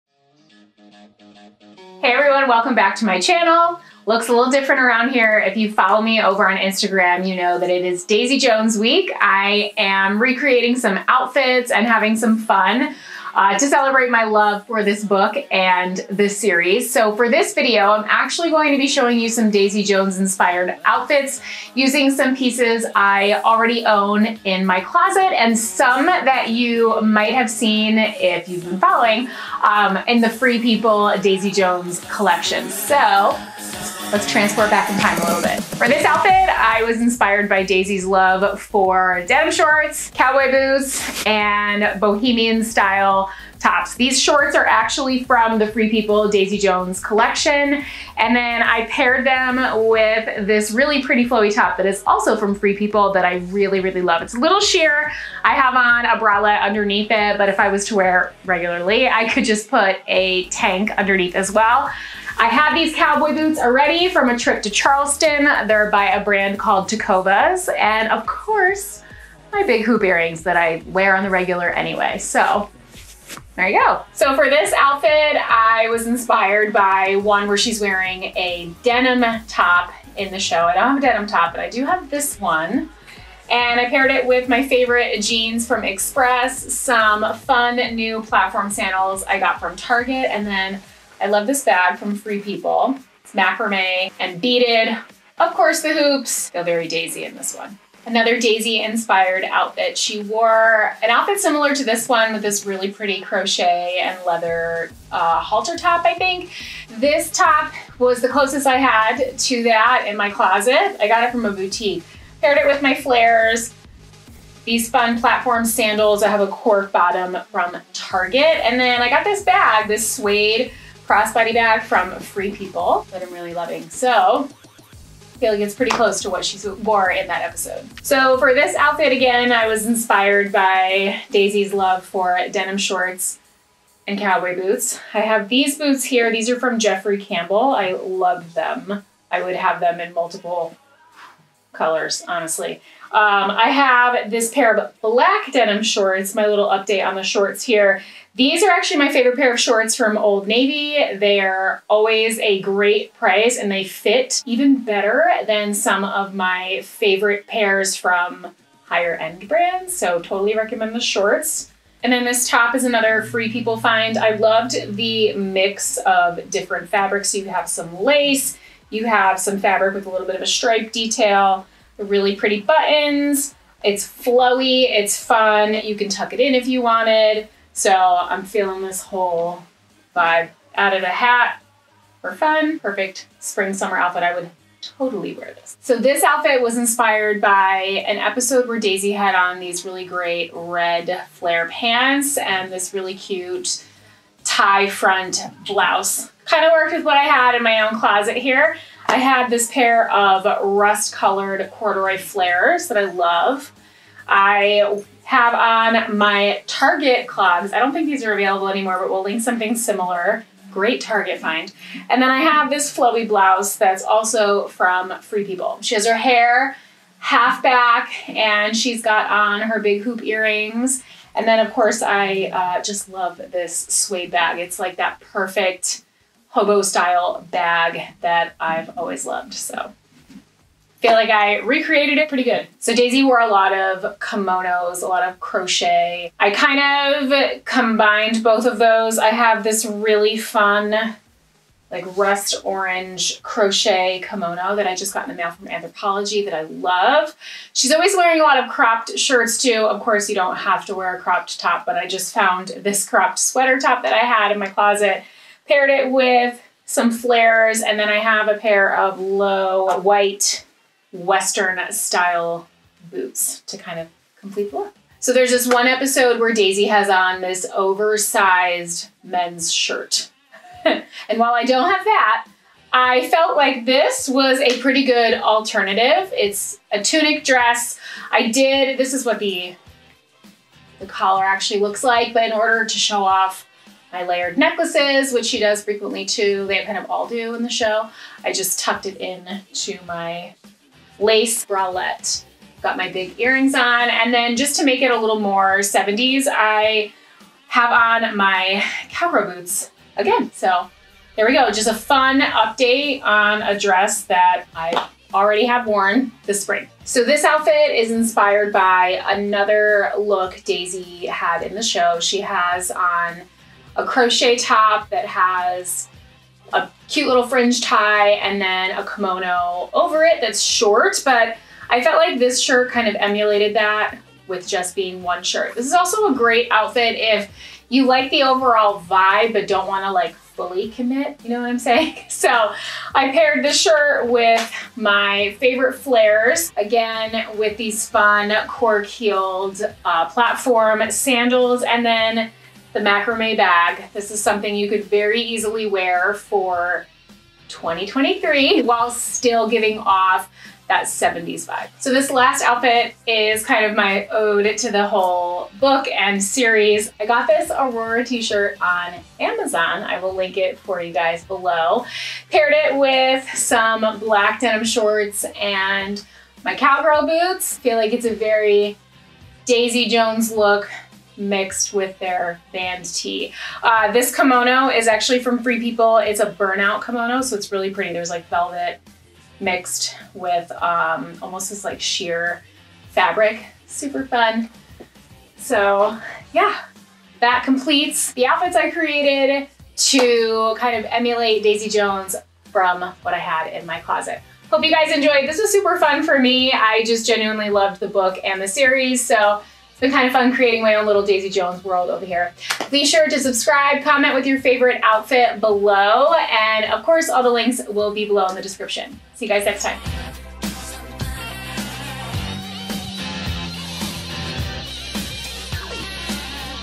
HUGE Summer 2023 Try On Haul | Free People, Boho Chic, 70s Retro Fashion | links in description
I hope you enjoy these 70's Boho Fashion Outfits for Summer 2023!! This is the end of Daisy Jones & The Six week but I had SO much fun doing these videos for you guys! Did you enjoy these outfits of the week? Should I channel another character for another series?

00:00 - Intro
00:37 - Outfit 1
01:15 - Outfit 2
01:38 - Outfit 3
02:08 - Outfit 4
03:06 - Outfit 5
03:54 - Outfit 6
04:25 - Outfit 7
05:07 - Outfit 8
05:53 - Outfit 9
06:27 - Outro